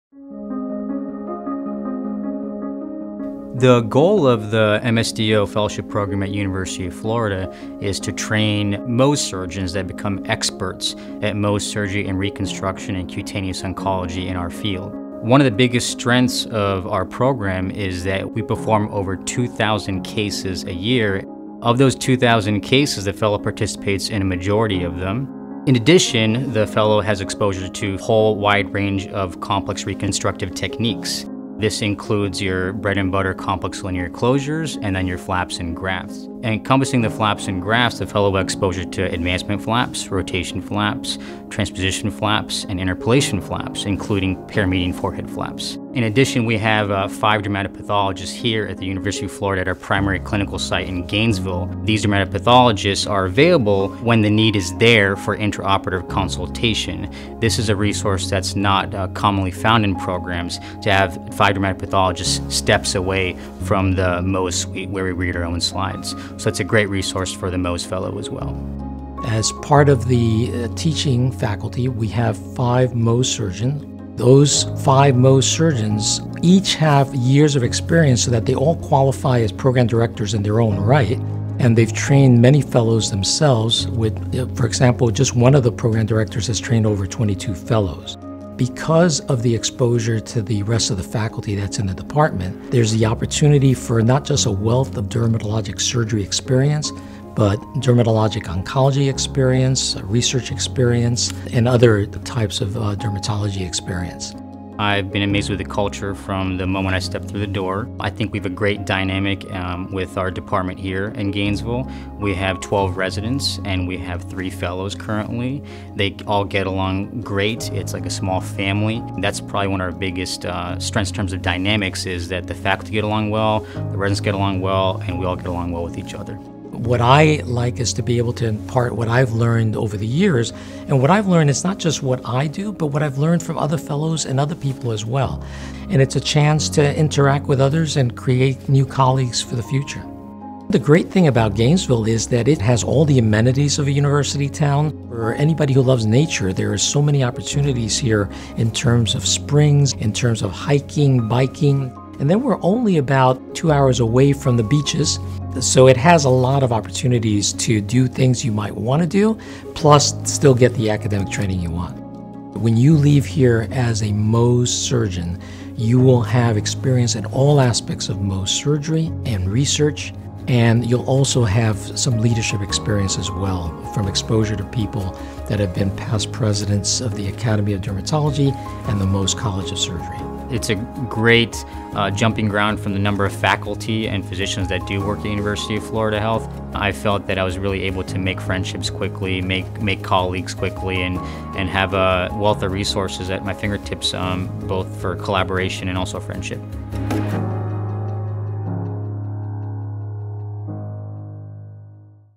University of Florida Micrographic Surgery and Dermatologic Oncology Fellowship Program
Learn about opportunities to train and work with the UF College of Medicine's Department of Dermatology at UF Health.